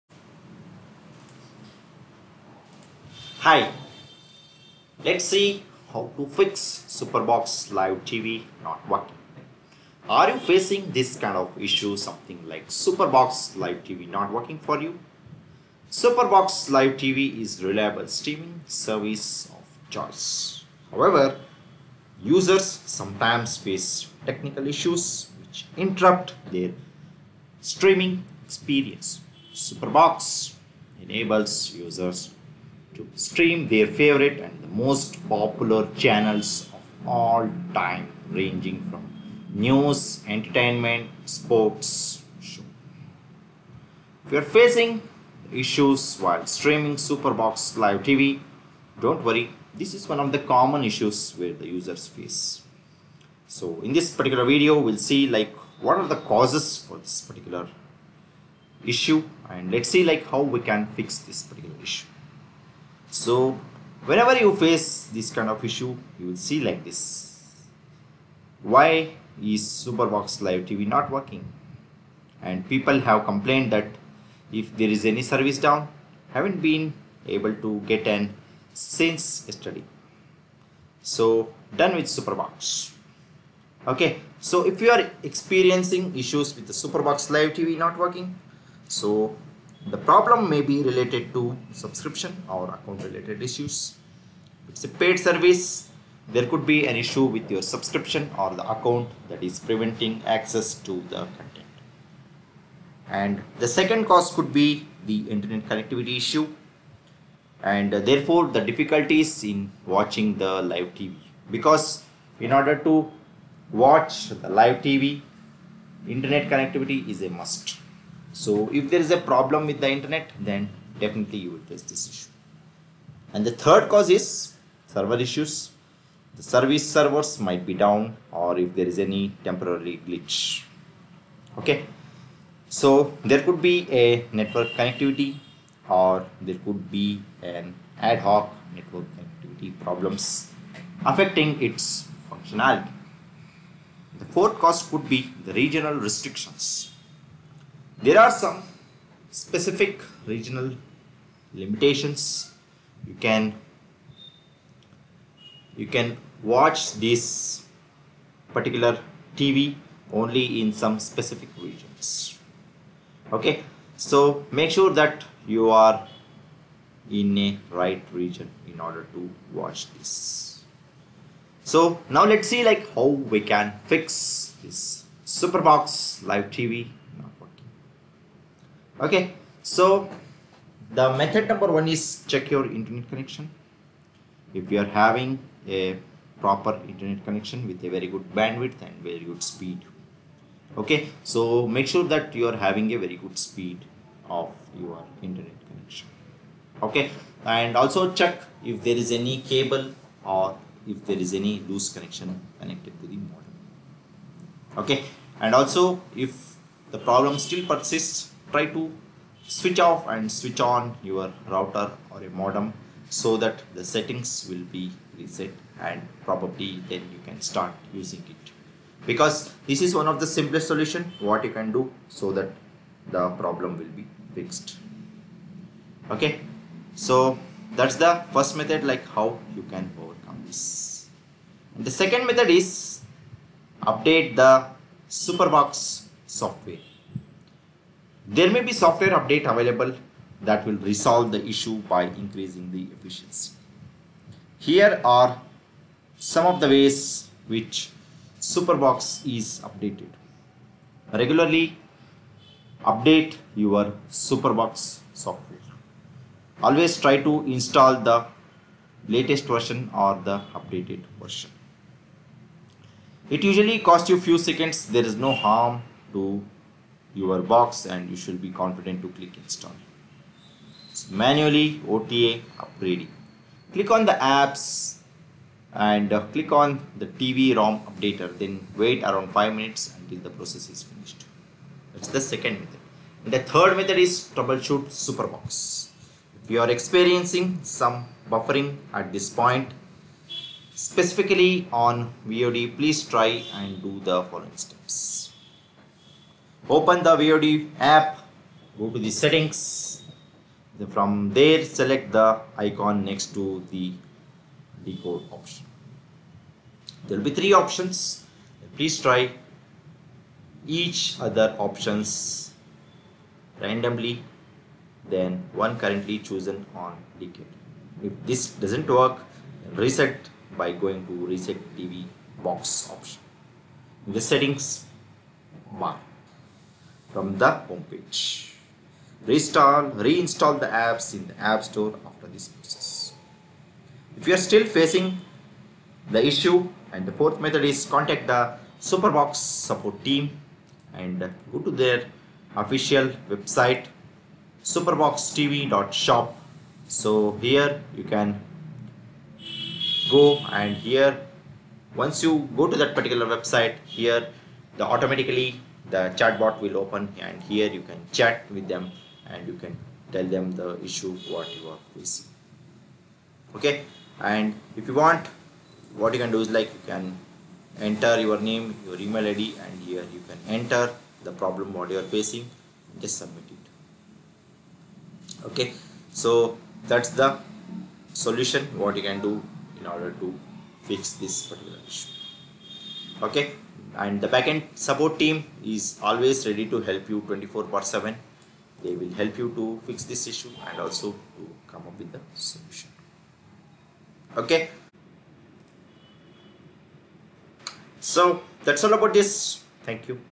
How to Fix SuperBox Live TV Not Working – Step-by-Step Troubleshooting Guide
Are you facing the issue of Superbox Live TV not working for you?Superbox Live TV is a reliable streaming service of choice.However, users sometimes face technical issues which interrupt their streaming experience.Superbox enables users to stream their favourite and most popular channels of all time ranging from news, entertainment, and sports show.If you are facing issues while streaming Superbox Live TV, do not worry, as this is a common issue users face.

0:00 Introduction
1:08 Why Is Superbox Live TV Not Working?
3:02 Solution 1: Check Your Internet Connection
3:52 Solution 2: Update The Superbox Software
4:44 Solution 3: Troubleshoot Superbox
5:37 Solution 4: Contact Superbox Customer Support